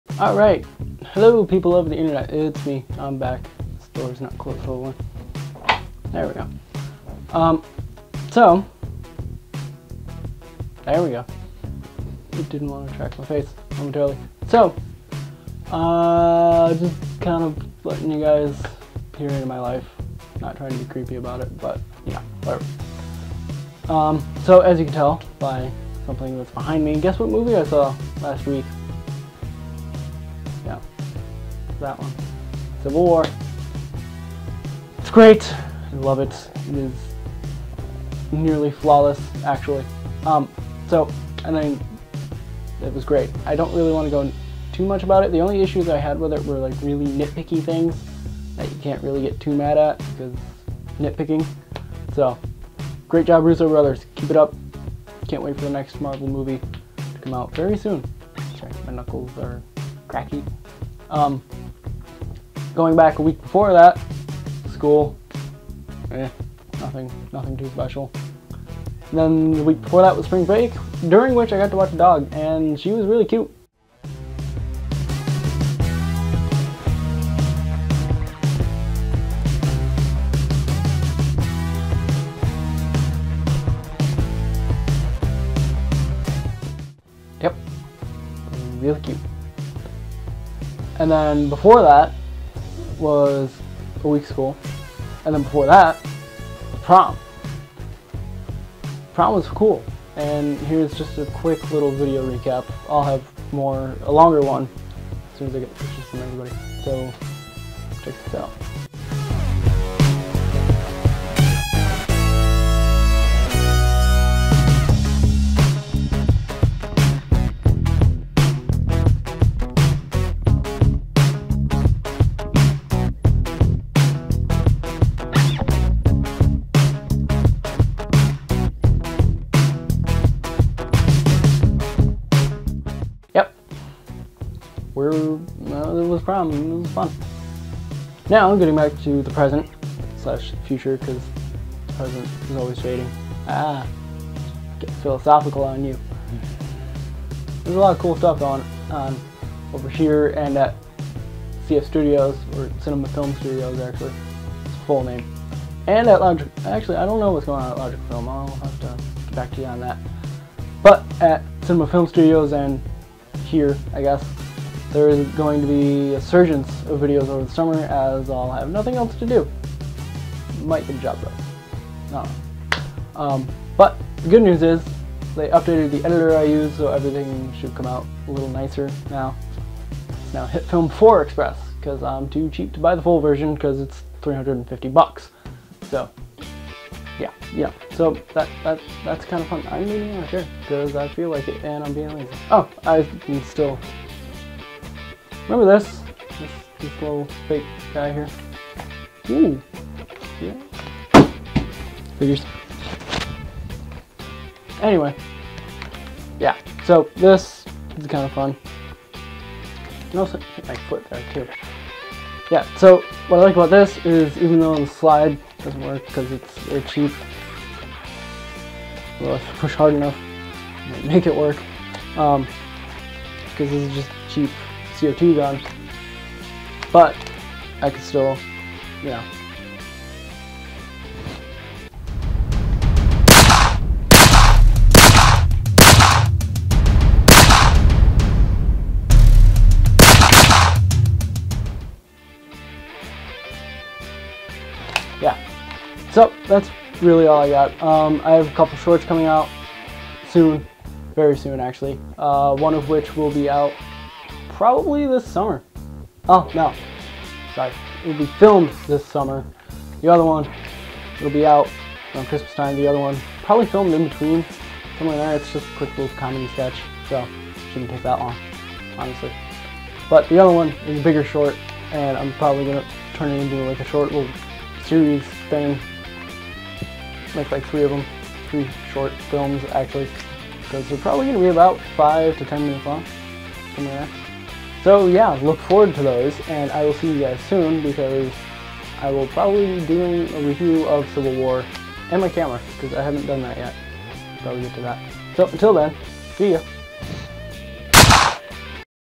Quick Life Recap(...and tiny Prom recap)
Just a Quick chat about recent events for me...

Tripod: Magnus VT-3000

Light: Interfit Softbox